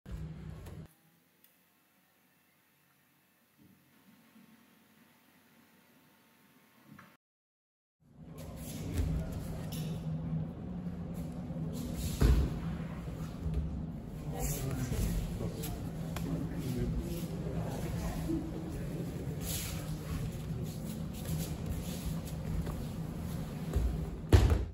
Aikidoa yli rajojen 2024. Harri Rautila 7. dan shihan
Aikidoa yli rajojen 2024 leiri Vantaan Jukara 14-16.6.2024.